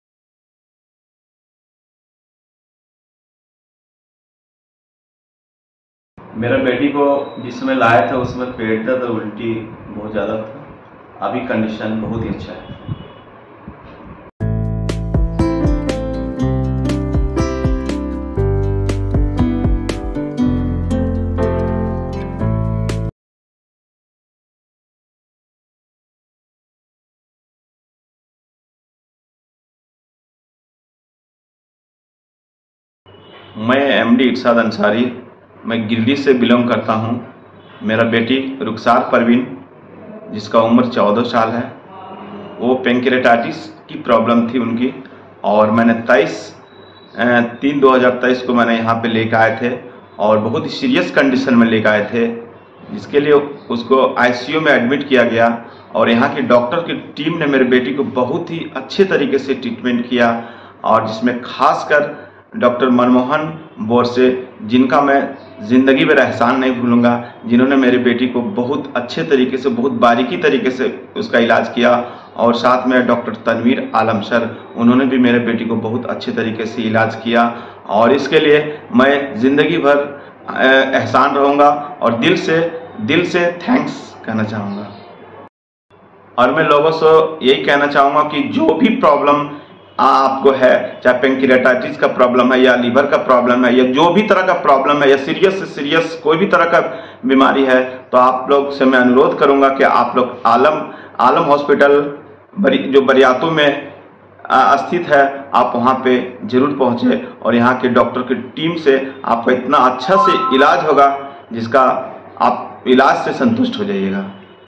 "14 वर्षीय बच्ची की पैंक्रिएटाइटिस से जुड़ी कहानी | आलम हॉस्पिटल राँची
For more info what's app us at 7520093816.
In this inspiring video, we bring you the remarkable journey of a brave 14-year-old girl who battled pancreatitis. Witness her incredible story of resilience and triumph as she receives expert treatment from the renowned Dr. Manmohan and Dr. Tanveer Alam at Alam Hospital Ranchi.

Pancreatitis can be a challenging condition, but with the expertise and compassion of our dedicated medical team, this young girl found hope and healing. Join us as we delve into the comprehensive care provided by Dr. Manmohan and Dr. Tanveer Alam, showcasing their unwavering commitment to restoring health and well-being.

Through this video, we aim to raise awareness about pancreatitis and highlight the exceptional medical care available at Alam Hospital Ranchi. Discover the cutting-edge techniques, personalized treatment plans, and compassionate approach that have made a significant difference in the lives of countless patients.

Witness the power of collaboration and the positive impact it can have on healthcare outcomes. This video serves as a testament to the extraordinary dedication and expertise of Dr. Manmohan and Dr. Tanveer Alam, as they work together to provide comprehensive care and empower individuals on their path to recovery.

Don't miss this inspiring video that celebrates the triumph of hope, resilience, and medical excellence. Join us on this incredible journey and be inspired by the remarkable story of a young girl's fight against pancreatitis.

Our Other Youtube Videos:
"14 वर्षीय बच्ची की पैंक्रिएटाइटिस से जुड़ी कहानी: डॉ. मनमोहन और डॉ. तनवीर आलम की उपचार सफलता"